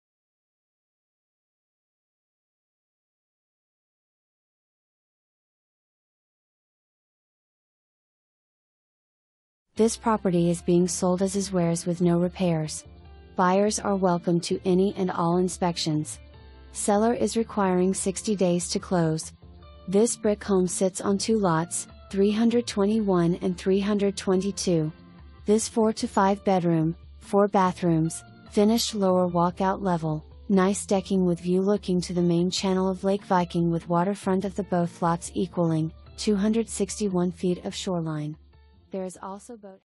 948 Lake Viking Terrace
948 Lake Viking Terrace
Gallatin, MO 64640

This property is being SOLD AS IS WHERE IS WITH NO REPAIRS. Buyers are welcome to any and all inspections. Seller is requiring 60 days to close. This brick home sits on 2 Lots, 321 & 322. This 4-5 bedroom, four bathrooms, finished lower walkout level, nice decking with view looking to the MAIN CHANNEL of Lake Viking with Waterfront of the both Lots equaling, 261 feet of shoreline. There is also boathouse and a dock. Detached Garage.

4 full bath

Nadine Parmenter
ReeceNichols Ide Capital Realty, LLC